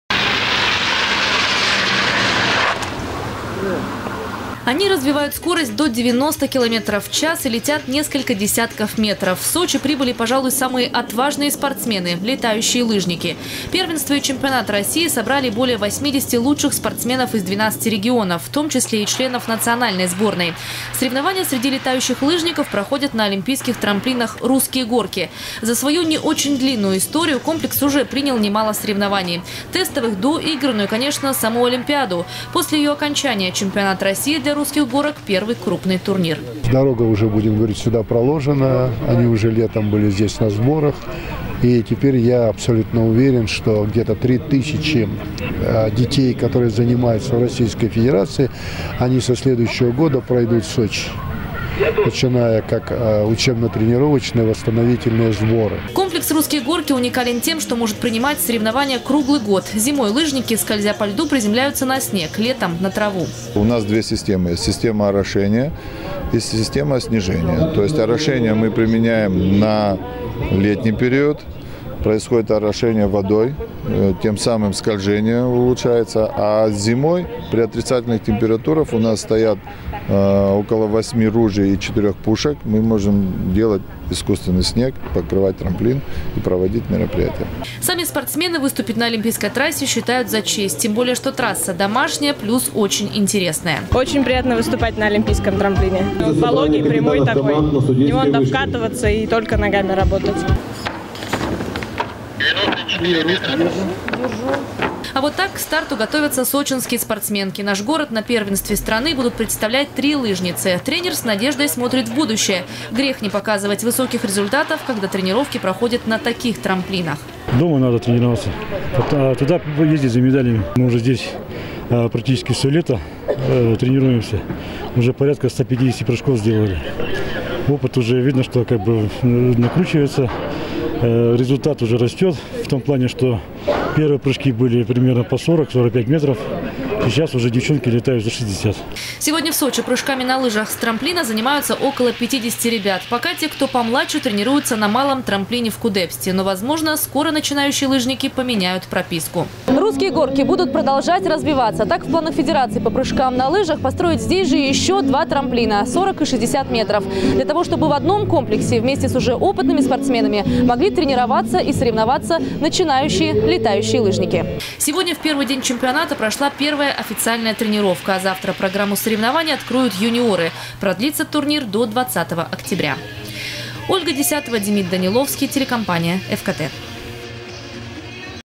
В Сочи съехались летающие лыжники из 12 регионов страны. Новости 24 Сочи
На курорте сегодня стартовал Чемпионат и первенство России по прыжкам на лыжах с трамплина. В нём участвуют более 80 спортсменов. В том числе члены российской сборной. Соревнования проходят на олимпийских трамплинах «Русские горки».
Сочи. Новости.
Телеканал Эфкате.